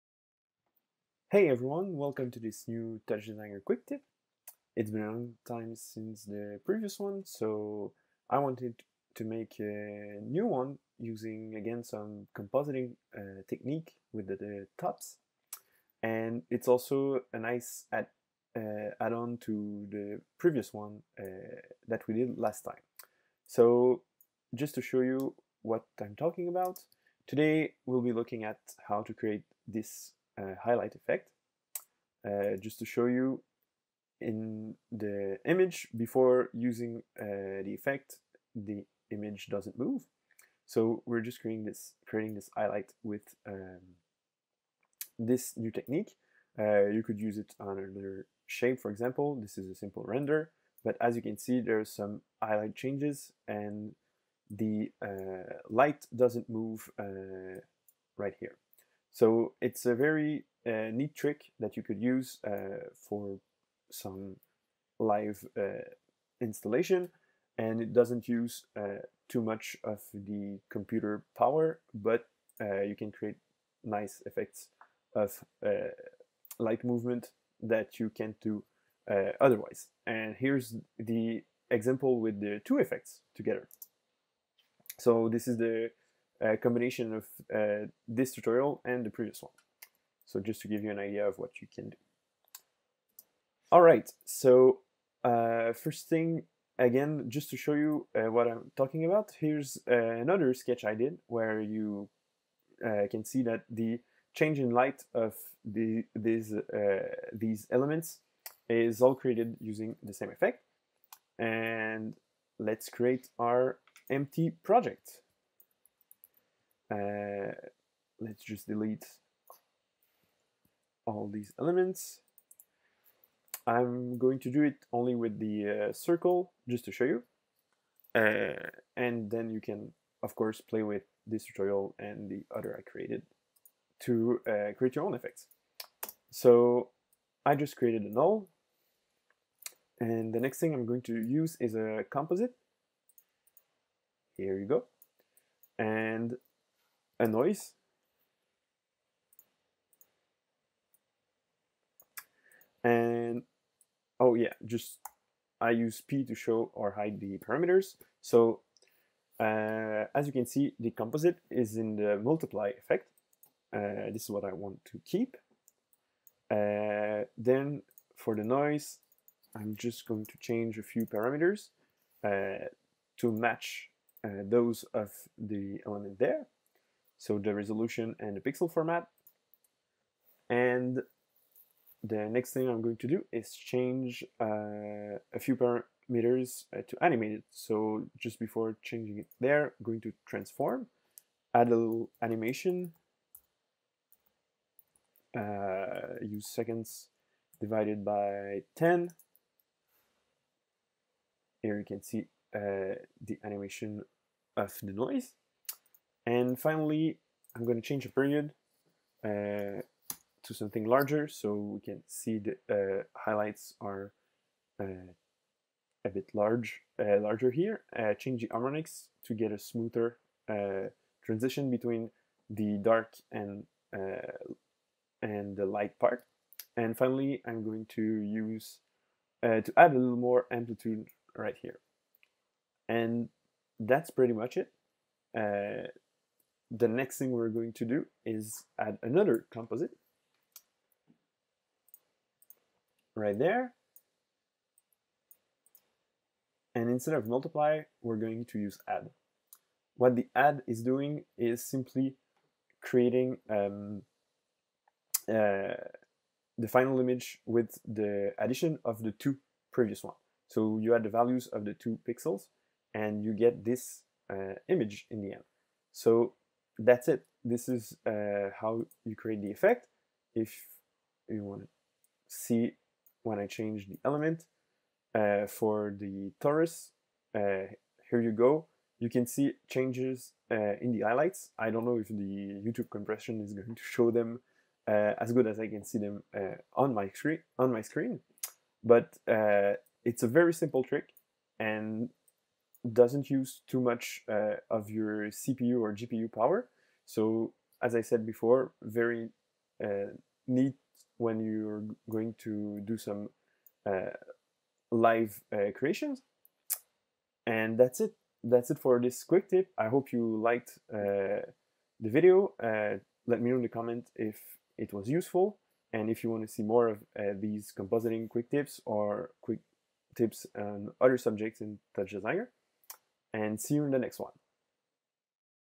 Light Movement Effect - Quick Tips TouchDesigner EN
In this video, we will look at how we can achieve a light movement effect using TOPs in TouchDesigner.

If you are interested in learning TouchDesigner from the basics, check out my class on skillshare